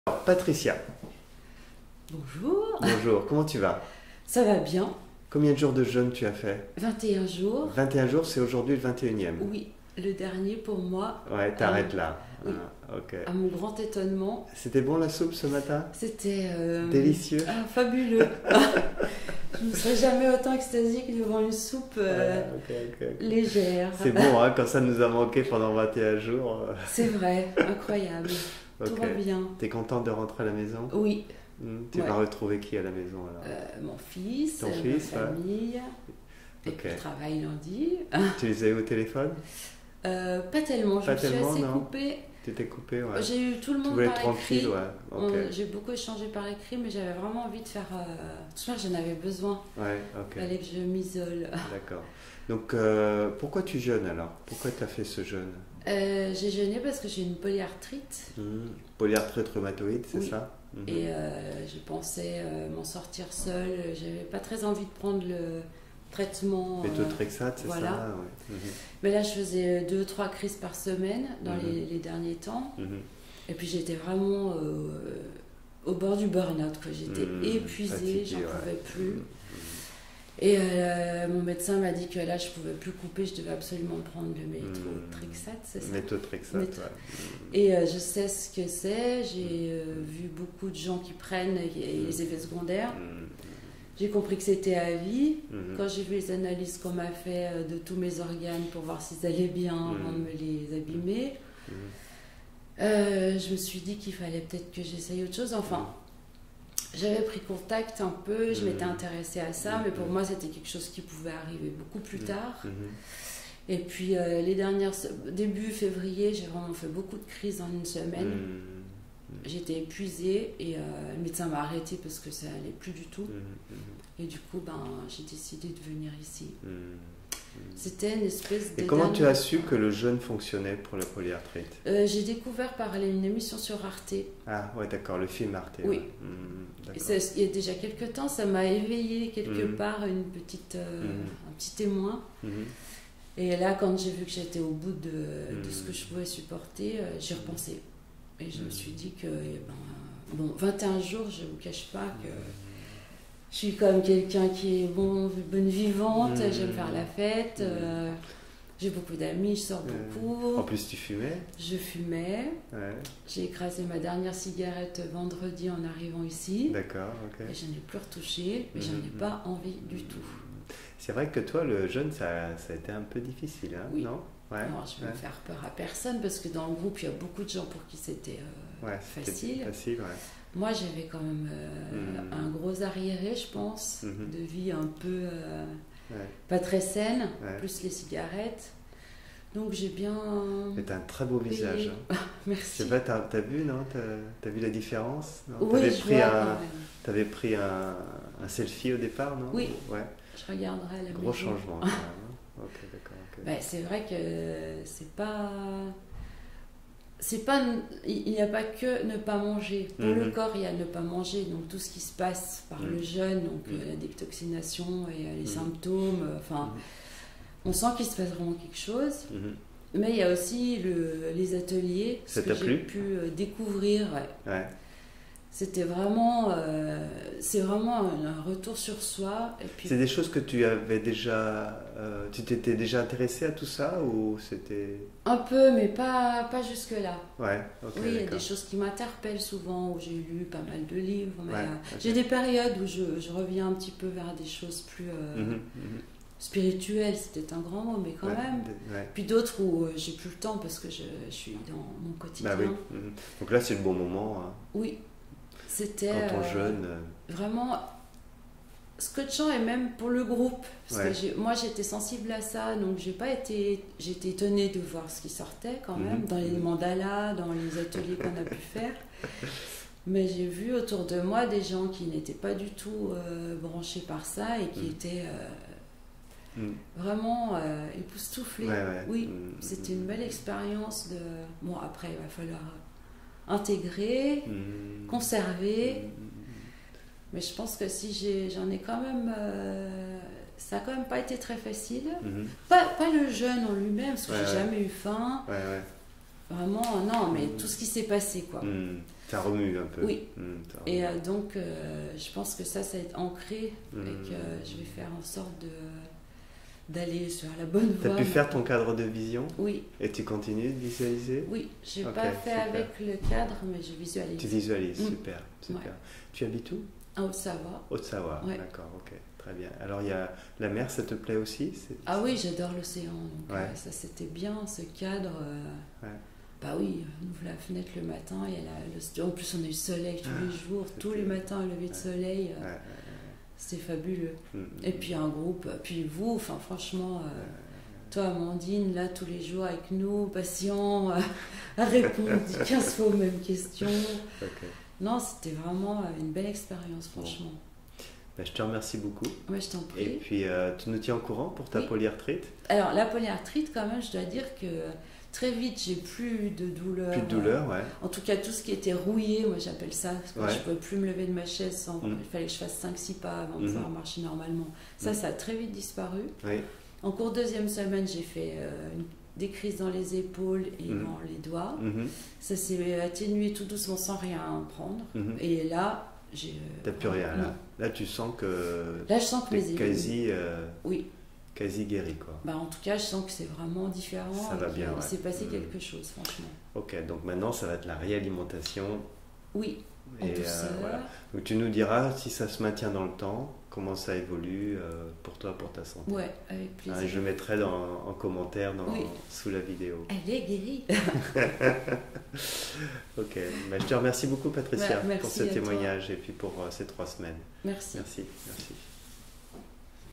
Les douleurs de polyarthrite rhumatoïde ont disparu en 21 jours de jeûne
Patricia vient de terminer 3 semaines de jeûne en groupe accompagné par Eric et Amandine. Elle a arrêté la cigarette avec facilité dès le premier jour de jeûne. Les douleurs de polyarthrite rhumatoïde ont disparu au fur et à mesure de la détoxination du corps. Pour elle, ce jeûne et les ateliers de développement personnel lui ont permis de faire un retour vers soi.